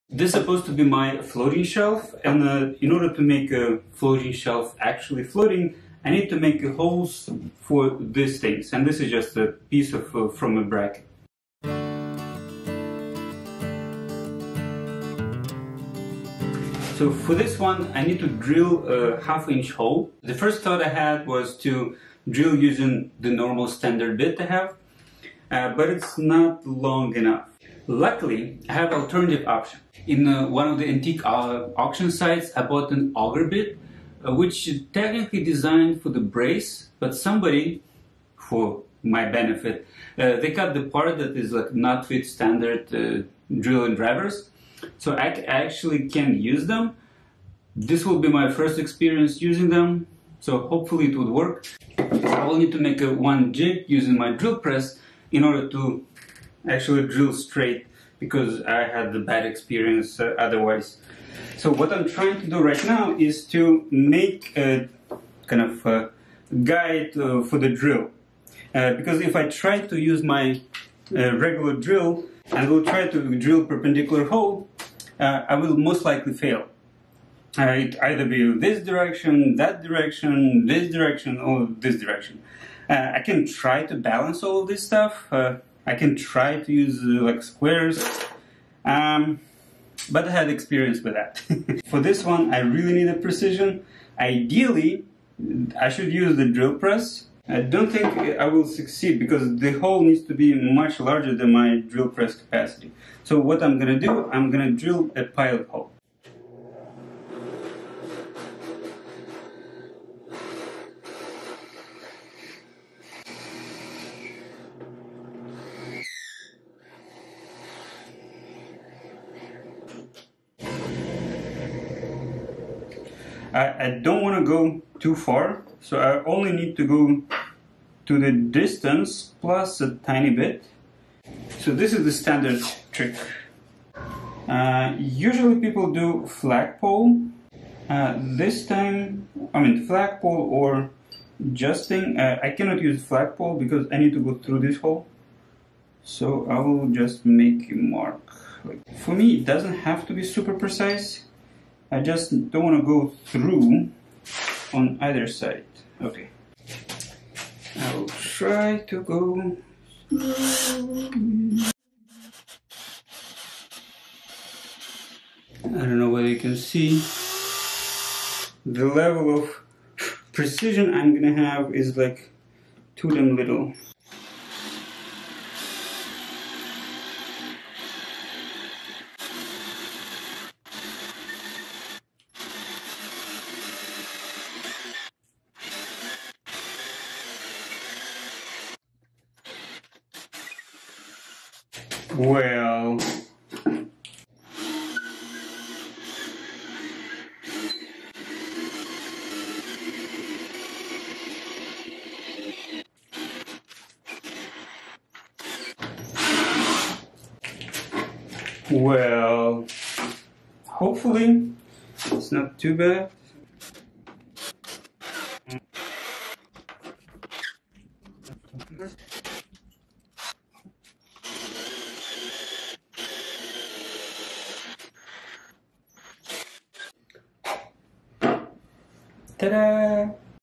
Drilling Straight Holes in Floating Shelves
A semi tutorial/semi report on how I drilled about 5" holes in the floating shelves I'm making. I don't have a drill press with this capacity, so had to use a simple jig.  I am also using a converted auger bit for the first time and quite like using it, compared to modern spiral bits.